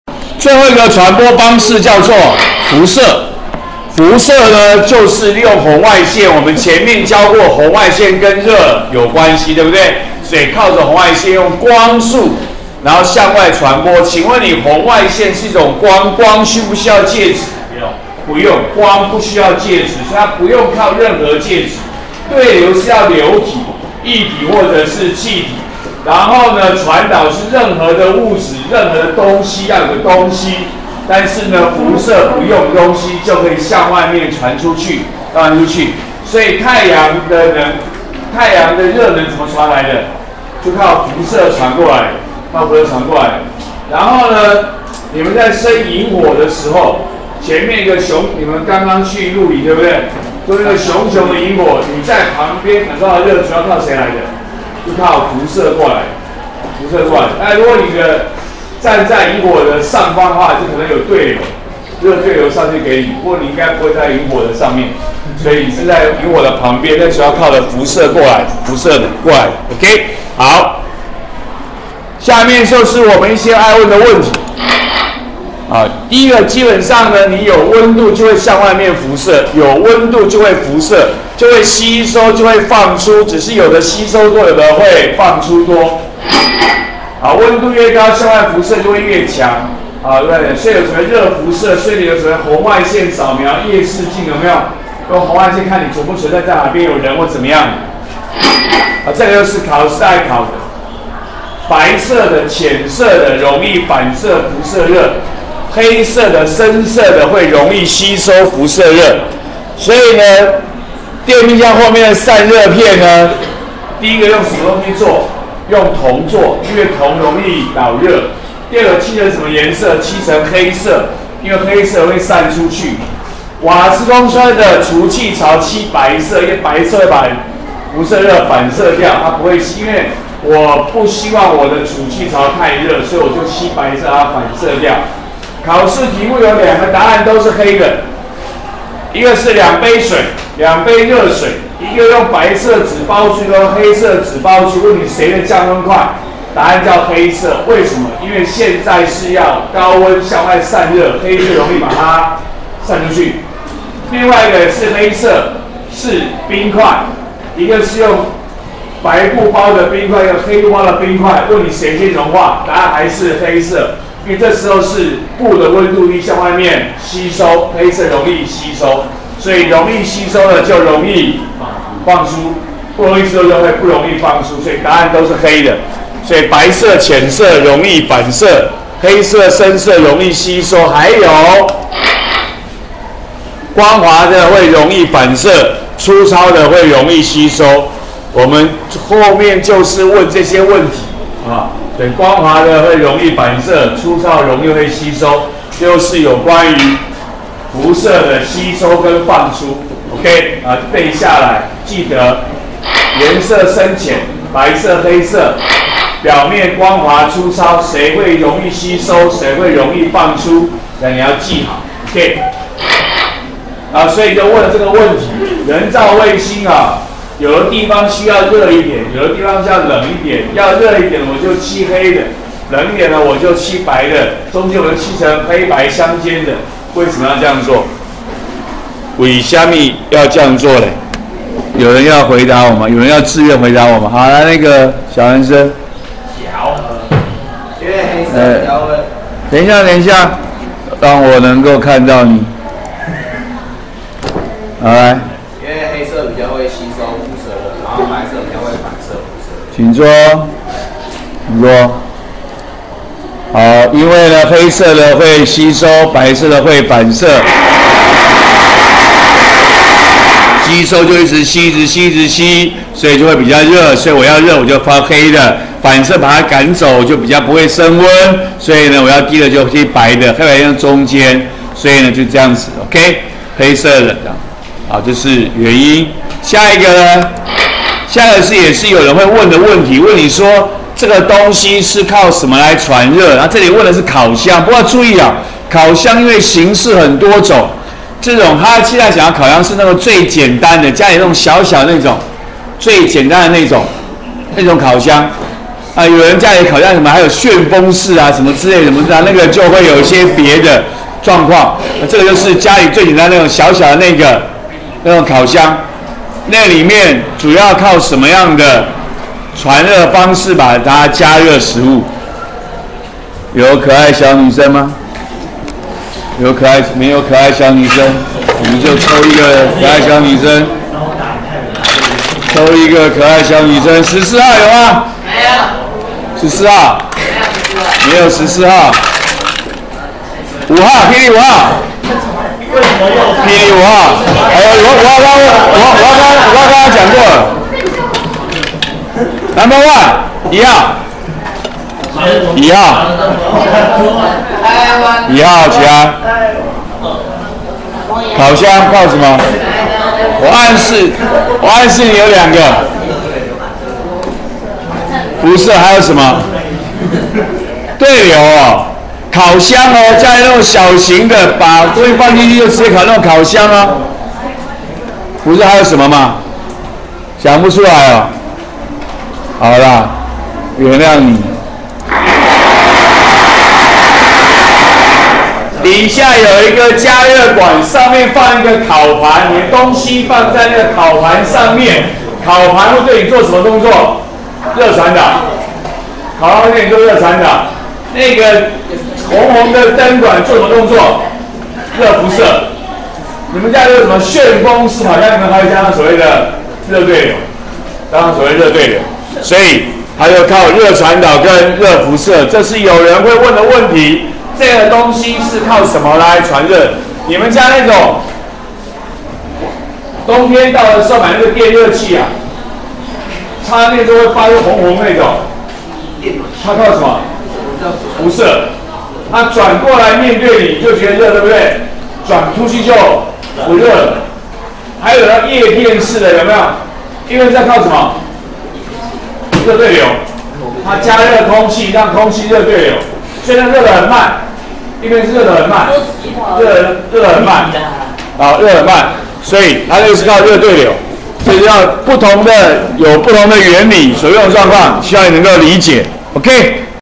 104學年國二理化＿溫度與熱26：輻射；動腦時間04、05
說明輻射內容，指出什麼情況容易吸收或反射輻射。動腦時間04問人造衛星不同部份，為何漆成黑色或白色。動腦時間05烤箱利用何種熱的傳播方式。